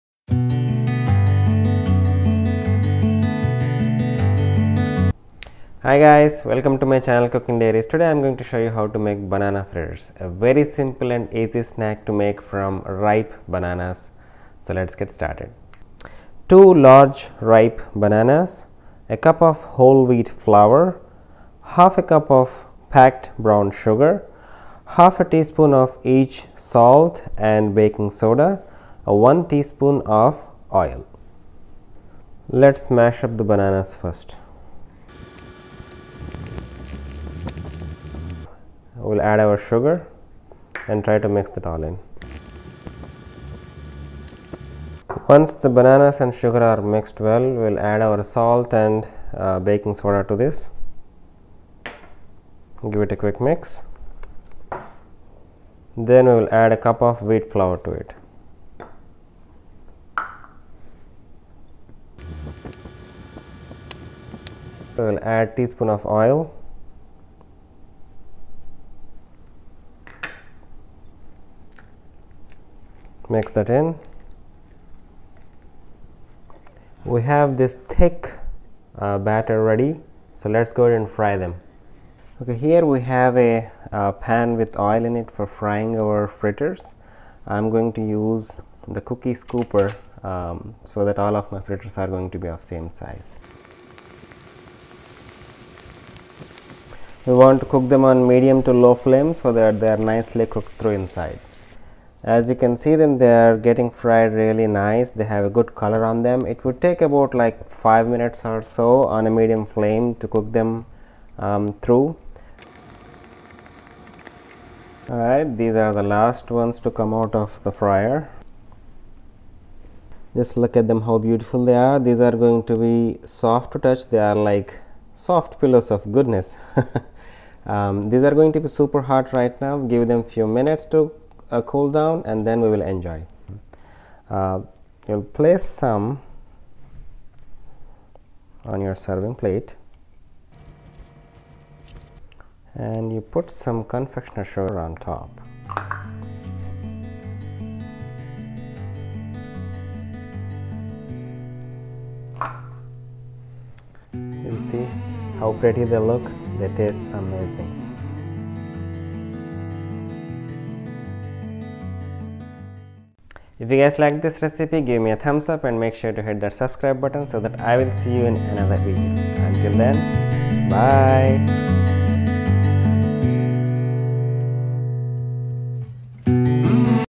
Banana Fritters!
Quick and easy snack that can be made from ripe bananas...Enjoy!